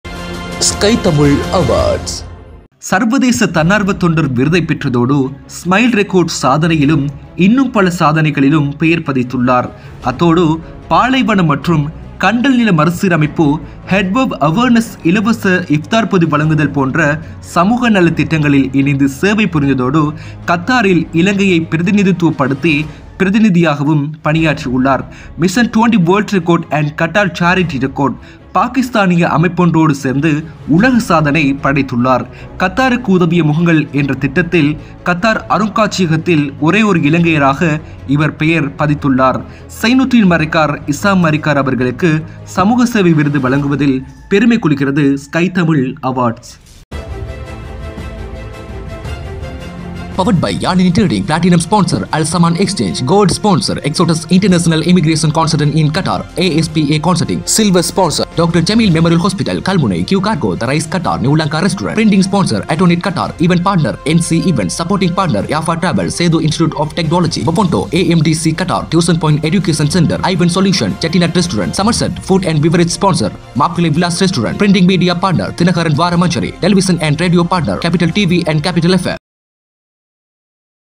Social Service Award 🇱🇰 | Isham Marikar | SKY TAMIL AWARDS 2023
Social Service Award  🇱🇰
Isham Marikar

SKY TAMIL AWARDS 2023 in Qatar Honor Sri Lankan and Indian Personalities

இலங்கை மற்றும்  இந்திய கத்தார் வாழ் ஆளுமைகளை கெளரவிக்கும் ஸ்கை தமிழ் விருது விழா

VENUE : Ashoka Hall , Indian Cultural Center -Doha